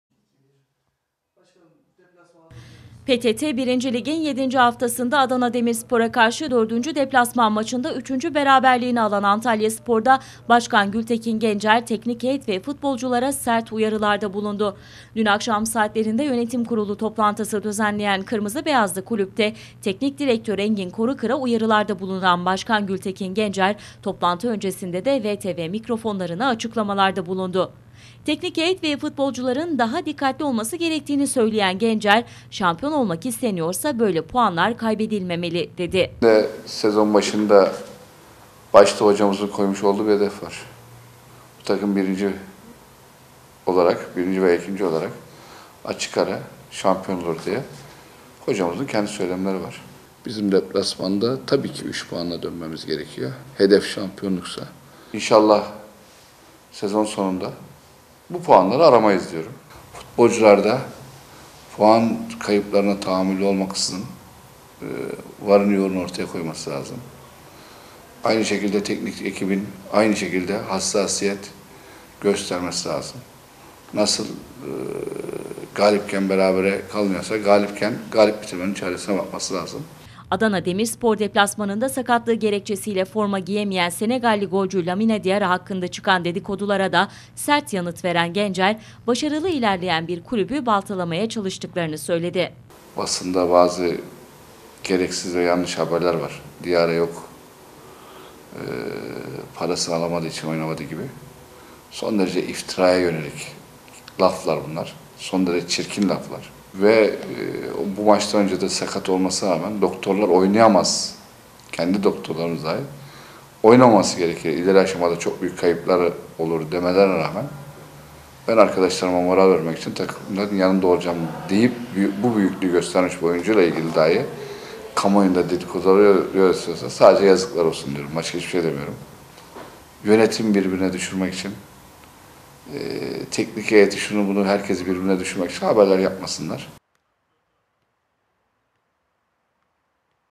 ANTALYASPOR BAŞKANI GÜLTEKİN GENCER SERT KONUŞTU
BAŞKAN GENCER'DEN TEKNİK HEYETE UYARI!

4. deplasman maçında 6. puanını kaybeden Antalyaspor'da Başkan Gültekin Gencer, teknik heyet ve futbolculara sert uyarılarda bulundu. Başkan Gencer, Diarra'nın parasını alamadığı için Adana Demirspor maçında forma giymediği dedikodularına da sert yanıt verdi

PTT 1. Lig'in 7. haftasında Adana Demirspor'a karşı 4. deplasman maçında 3. beraberliğini alan Antalyaspor'da Başkan Gültekin Gencer, tenik heyet ve futbolculara sert uyarılarda bulundu. Dün akşam saatlerinde yönetim kurulu toplantısı düzenleyen Kırmızı-Beyazlı kulüpte Teknik Direktör Engin Korukır'a uyarılarda bulunan Başkan Gültekin Gencer, toplantının öncesinde de VTV mikrofonlarına açıklamalarda bulundu.
Teknik heyet ve futbolcuların daha dikkatli olması gerektiğini söyleyen Gencer, "Şampiyon olmak isteniyorsa böyle puanlar kaybedilmemeli" dedi.

Adana Demirspor deplasmanında sakatlığı gerekçesiyle forma giyemeyen Senegalli golcü Lamine Diarra hakkında çıkan dedikodulara da sert yanıt veren Gencer, başarılı ilerleyen bir kulübü baltalamaya çalıştıklarını söyledi.